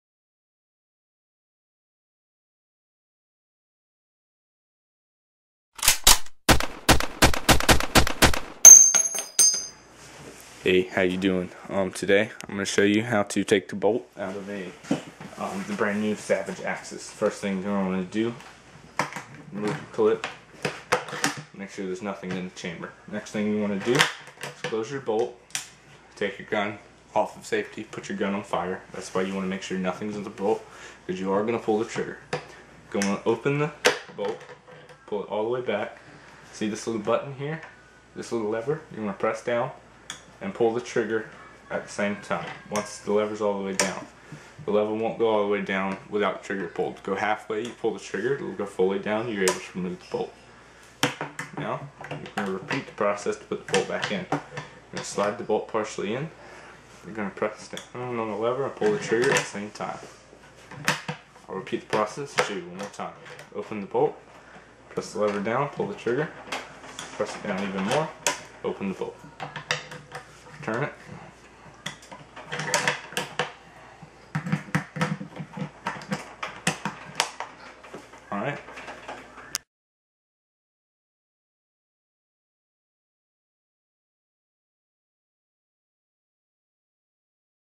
Savage Axis: How to remove bolt from the savage axis
In this tutorial I will show you how to remove the bolt from the savage axis and how to put it back in.